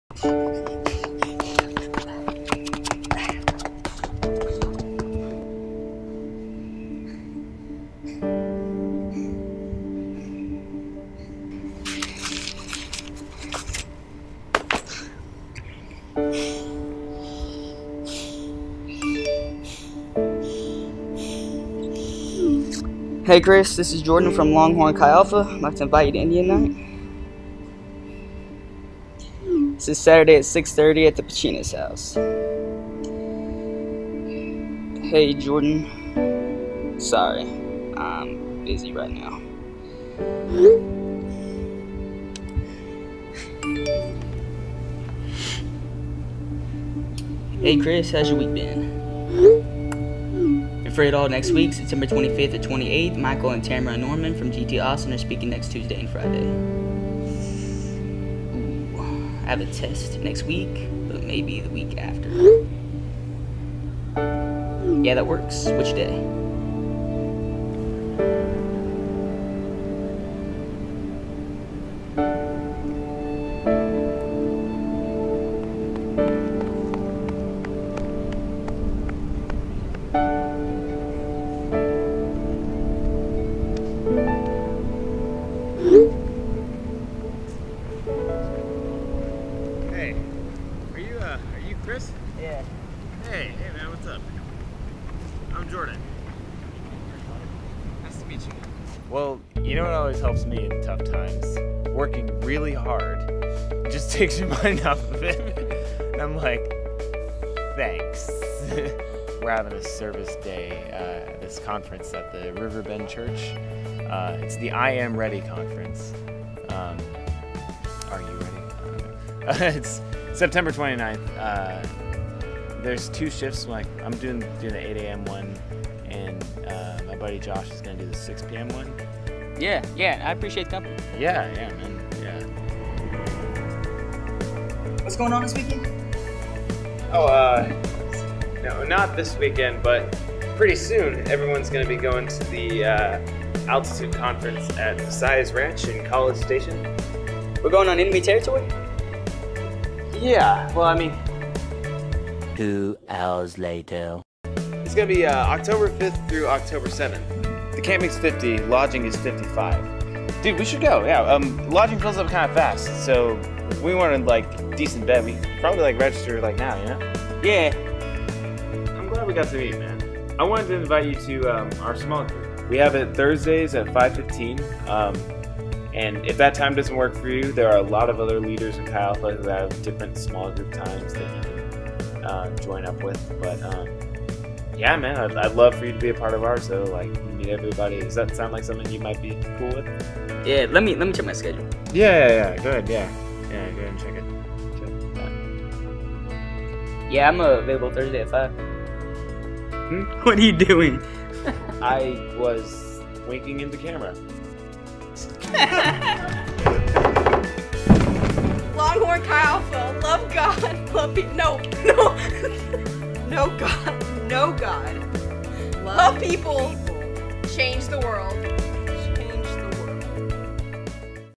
Work Really Hard
Jordan pursues a freshman for his small group whom he hasn't met in person yet. His name is Chris.